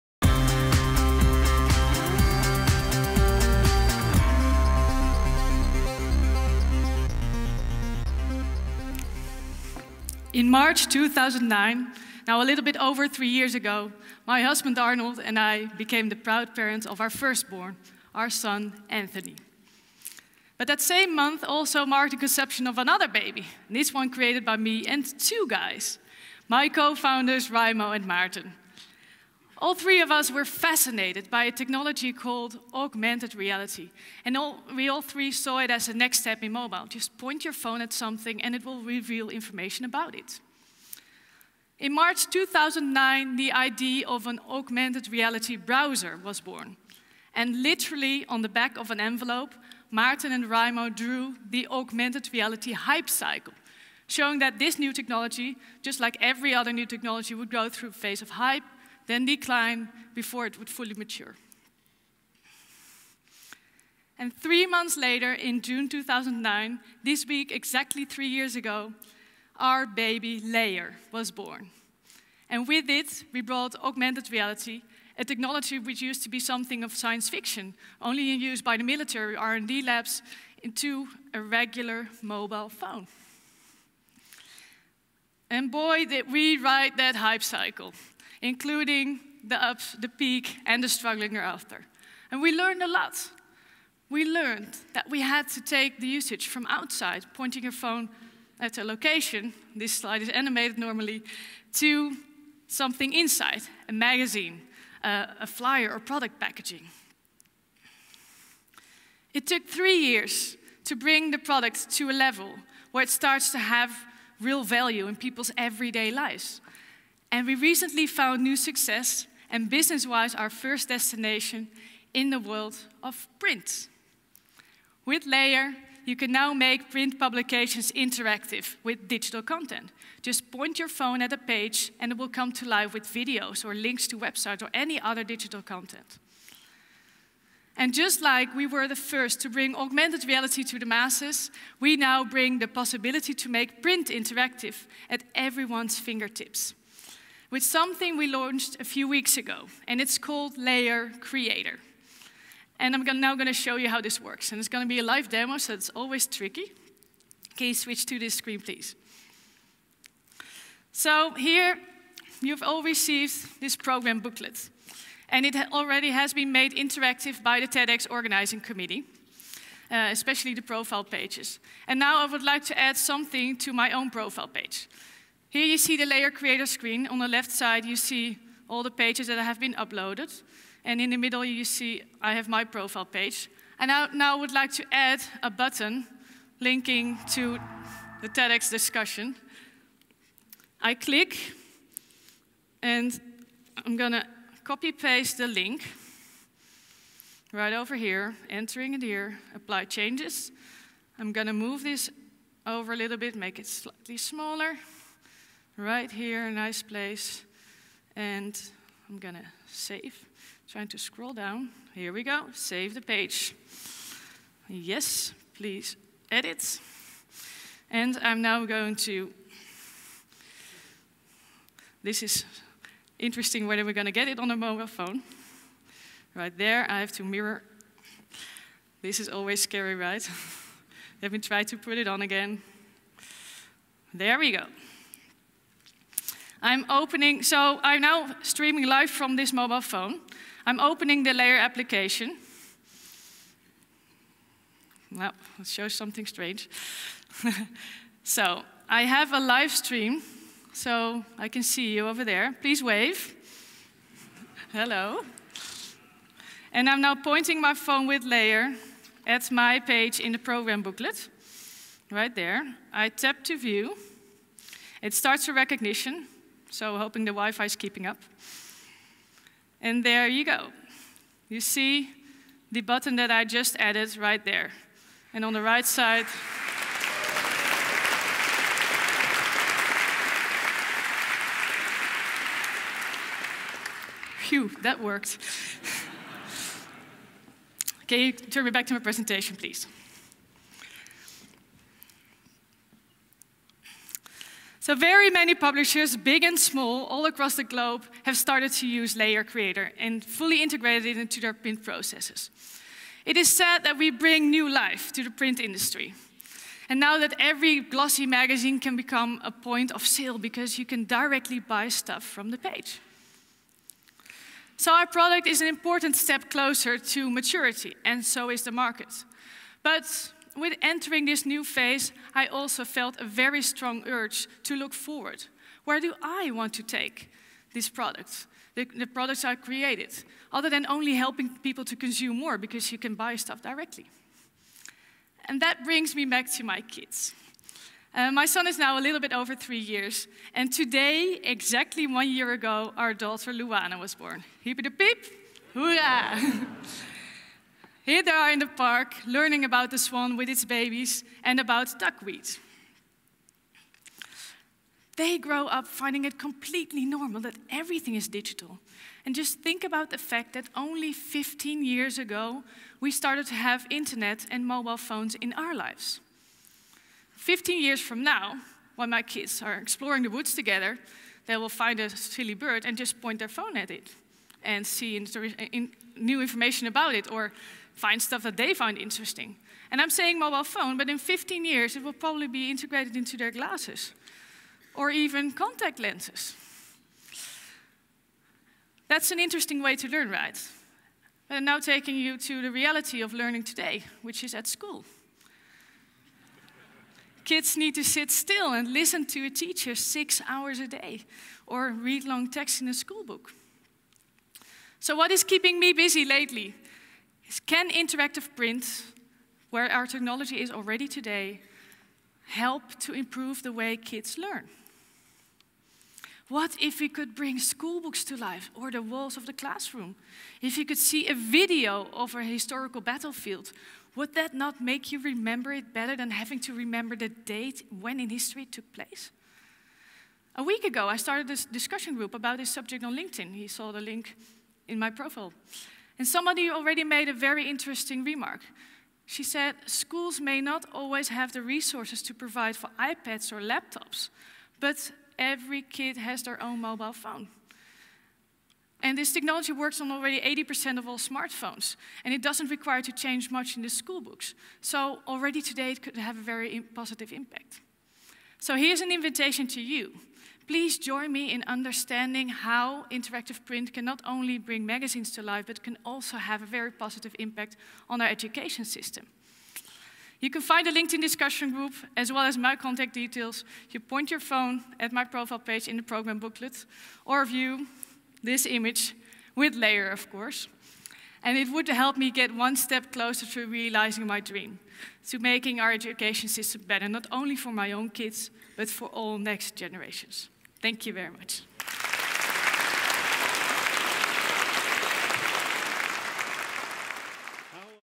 Using interactive print for better education: Claire Boonstra at TEDxBinnenhof 2012
Claire Boonstra (Heemskerk) is co-founder of Layar. She holds an MSc in Civil Engineering, (TUDelft), worked with KPN, Unilever and Talpa Digital, was co-organizer of Mobile Monday Amsterdam and Managing Director of SPRXmobile. At Layar, Claire is responsible for the Business Development and helps publishers to make their print publications interactive. Claire was recently elected as a 'Young Global Leader' of the World Economic Forum, was a 'Rising Star' at the Women's Forum for the Economy and Society, was Online Mediawoman of the Year, is 'Young Advisor' of EU Commissioner for the Digital Agenda Neelie Kroes and has been listed several times as one of the 'most influential women in technology'. She is married and has two little kids.

@ TEDx Binnenhof 2012